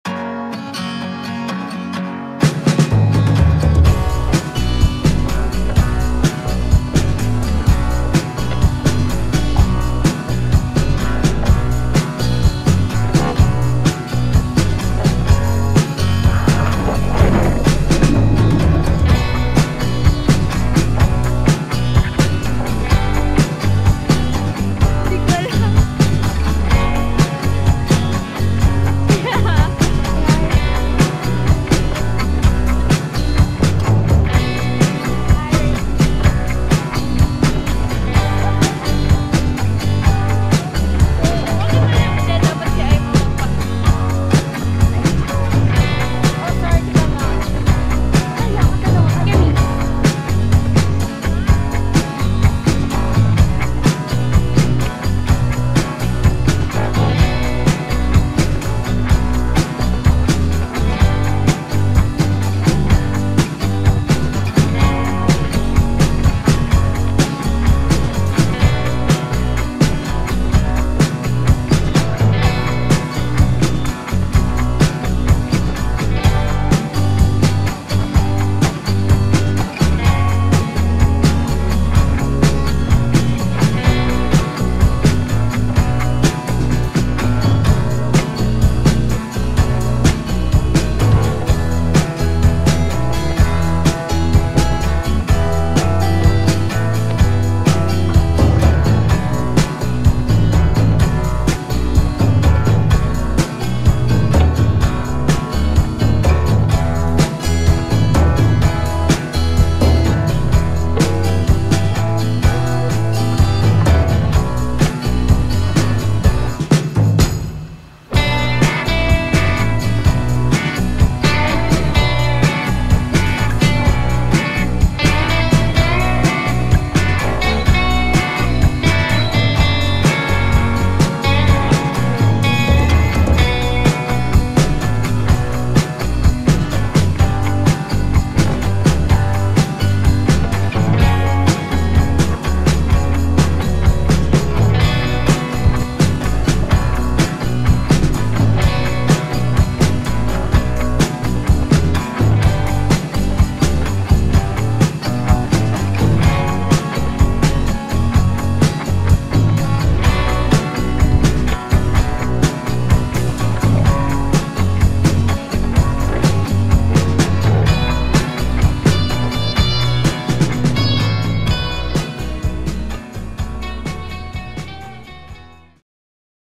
Lakeside Park Whistler
The last day of our 3-day vacation in Whistler. Drone flying early in the morning and just chilling out at the lake